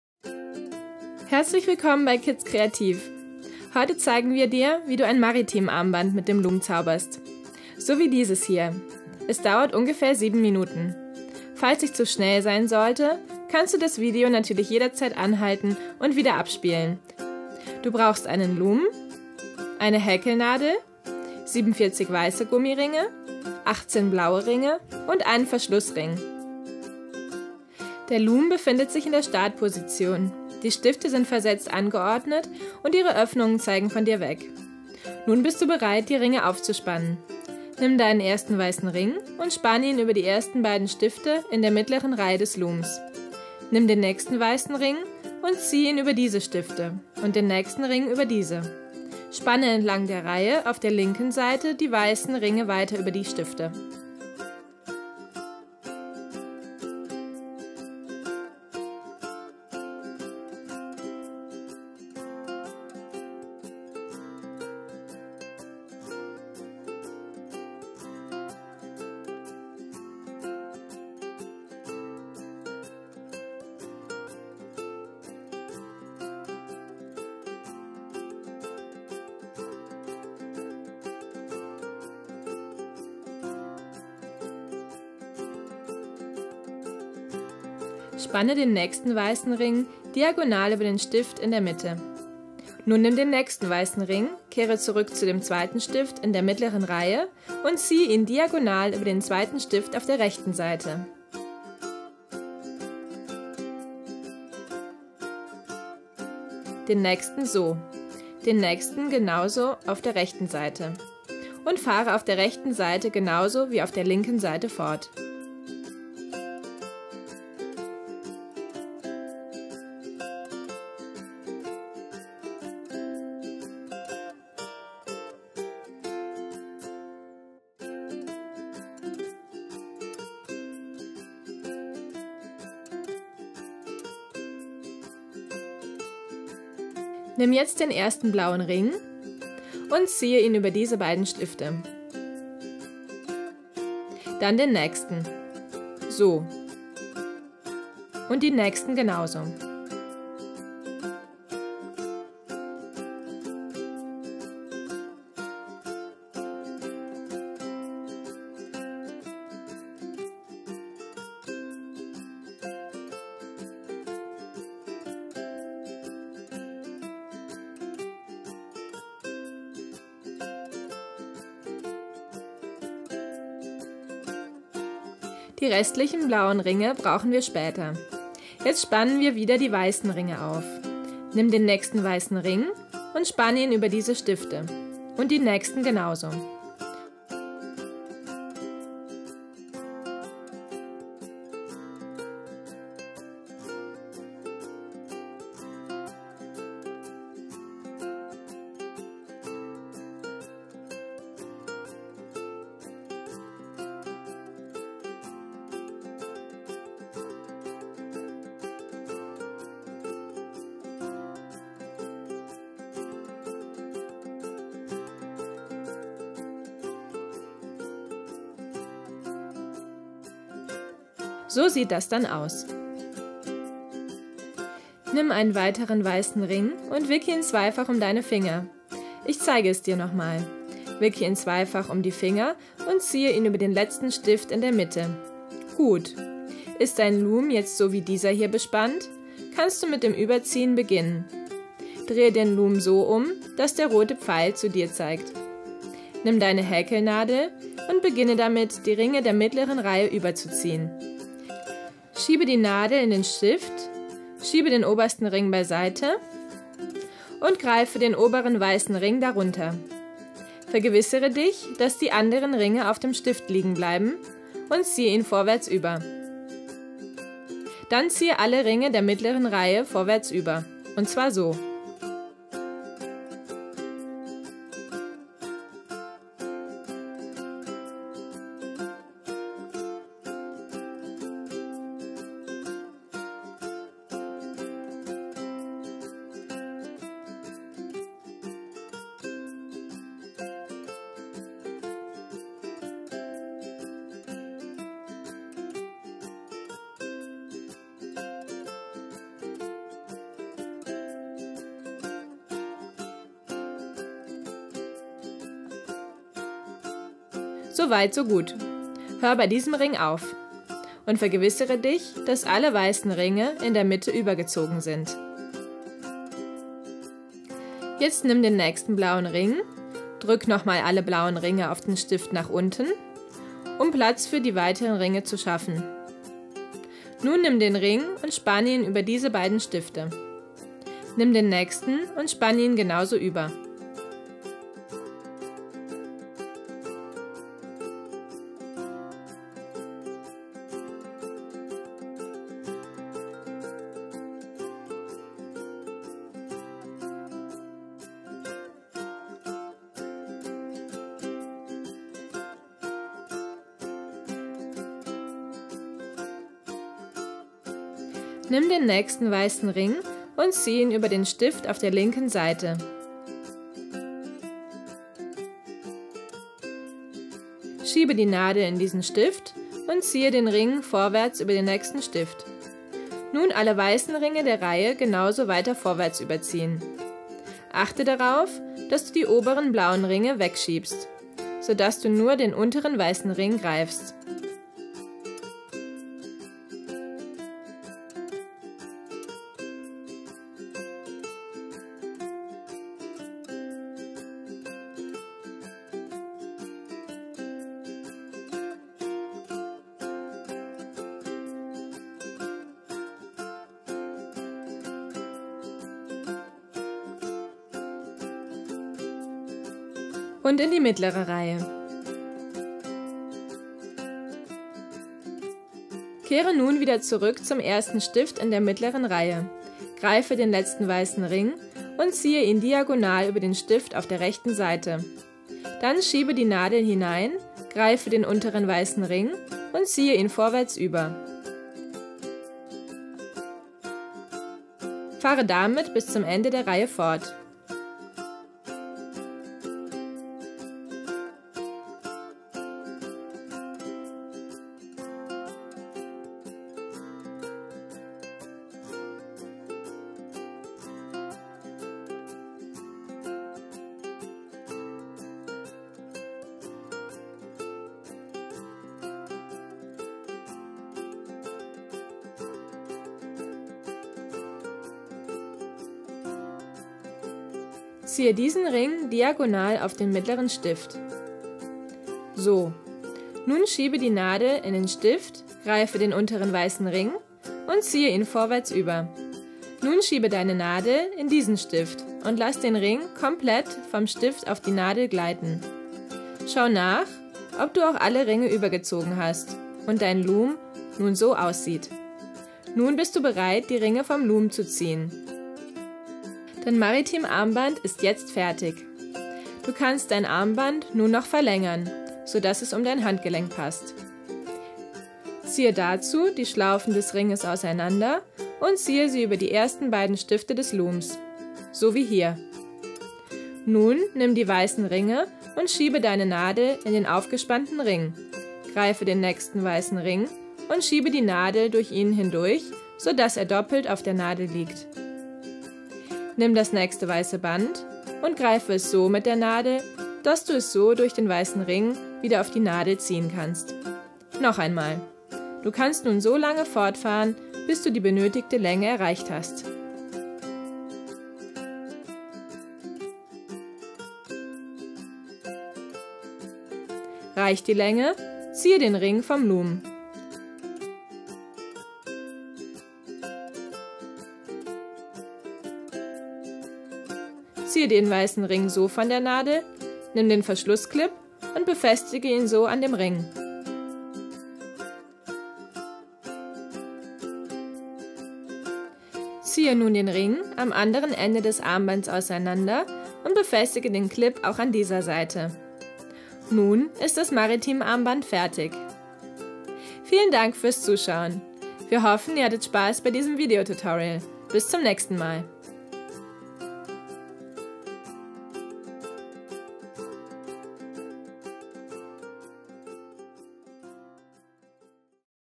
Kids kreativ Loom-Anleitung: Maritim gestreift
Das süße Loomband im maritimen Style ist der absolute Hingucker! Mehr coole Ideen findet Ihr in unseren Kids Kreativ Magazinen